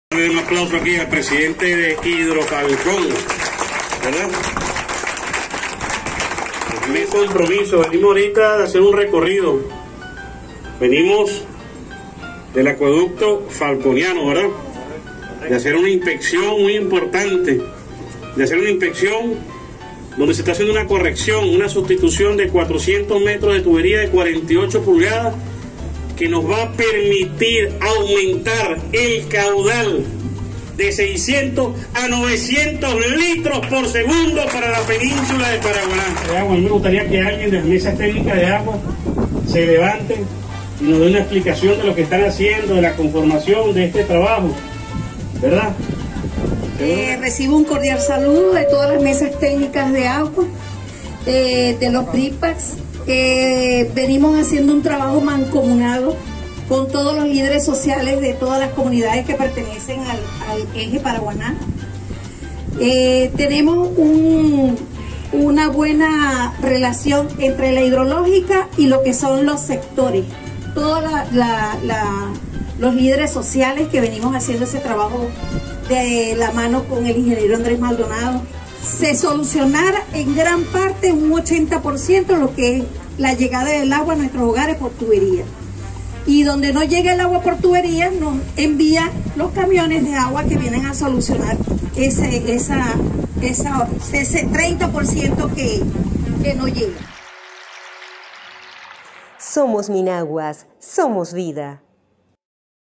Caudal de agua hacia Paraguaná se incrementará a 900 litros por segundo
El suministro de agua potable hacia la península del estado Falcón se aumentará en 300 litros por segundo, una vez culminadas las obras desarrolladas en el acueducto falconiano

El ministro del Poder Popular de Atención de las Aguas (MinAguas), G/D Rodolfo Marco Torres, inspeccionó las labores de sustitución de 400 metros de tubería de 48 pulgadas en el acueducto de la Península de Paraguaná, que permitirá sumar a la producción de agua 300 litros por segundo, pasando de 600 a 900 litros, destinados a los municipios Carirubana, Falcón y Los Taques y el Centro de Refinación Paraguaná, en beneficio de más de 320 mil habitantes.

“En la península de Paraguaná hemos venido a supervisar la sustitución de 400 metros del acueducto falconiano de 48 pulgadas de diámetro, donde además se han efectuado corrección de fugas y averías, por ello quiero felicitar a la fuerza trabajadora de Hidrofalcón que ha hecho posible este extraordinario trabajo”, expresó el ministro Marco Torres.

“Hemos avanzado mucho y estoy seguro que vamos a seguir consolidando el sistema hídrico en el estado Falcón, gracias al apoyo de nuestro presidente Nicolás Maduro, que, a pesar de las dificultades, a pesar del bloqueo, la guerra económica y la derecha fascista, ahí está un Presidente obrero, guerrero y batallador pendiente de su pueblo”, subrayó el Ministro, desde el acueducto.

Por su parte el ingeniero Andrés Maldonado, presidente de Hidrofalcón, agradeció al Ejecutivo Nacional que encabeza el Presidente de la República, Nicolás Maduro, por esta obra que permitirá mejorar el caudal hacia Paraguaná y que estabilizará el sistema para avanzar hacia la confiabilidad, apoyados en el Plan Nacional del Sistema Hídrico.